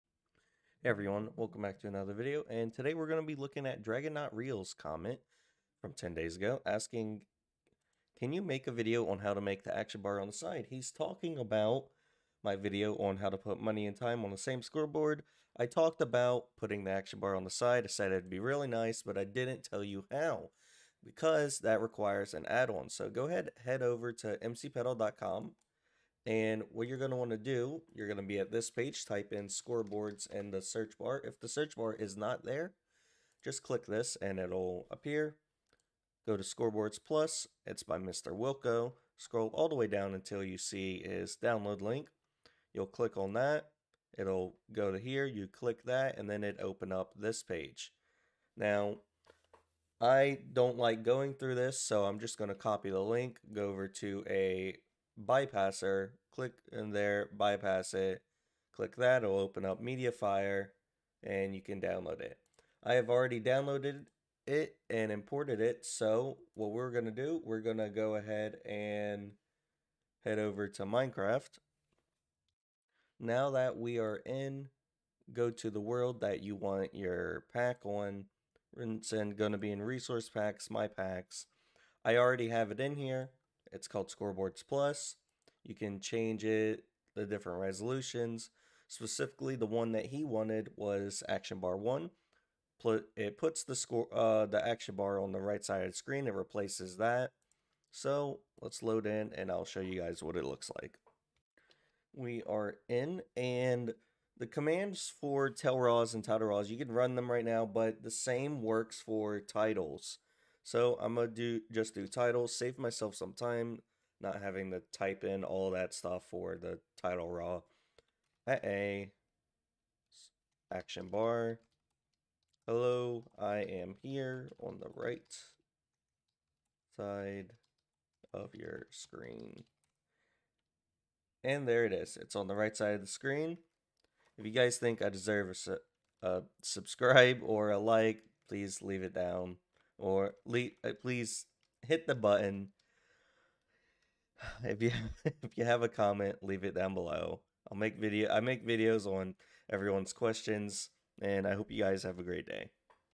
How to put the Action bar on the right side of your screen
In todays video Stormy tells you guys about the resource pack that allows the action bar to be on the right side of your screen. Super useful for custom scoreboards with titleraw.

XBOX - NotSerFrost
Microsoft banned my other accounts. Still won't tell me why.